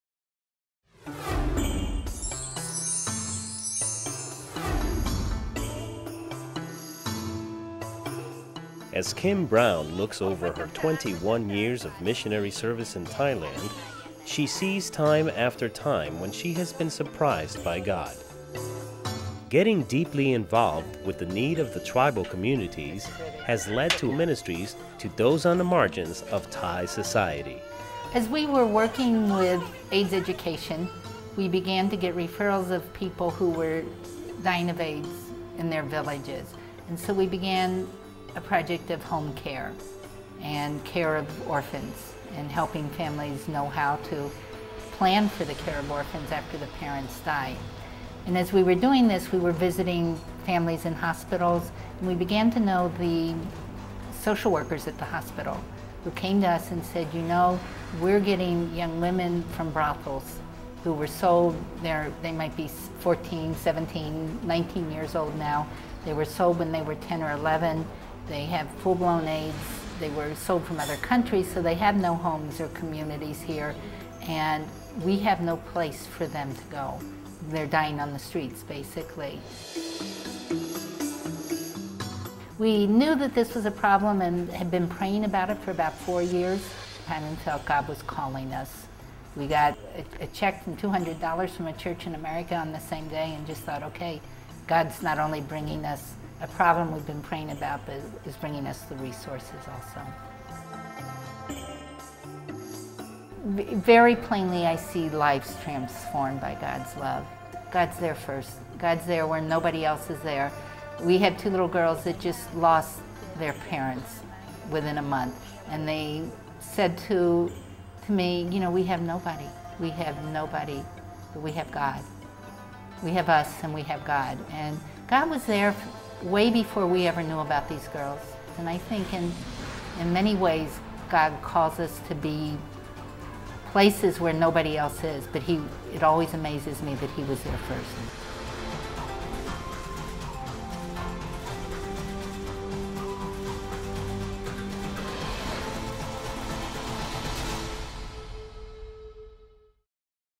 God at Work - Kim Brown
Kim works with tribal peoples in Thailand who are coping with the problems caused by AIDS, disabilities, and migration from their traditional villages to urban slums. The Thailand Karen Baptist Convention and American Baptist International Ministries sponsor her ministries.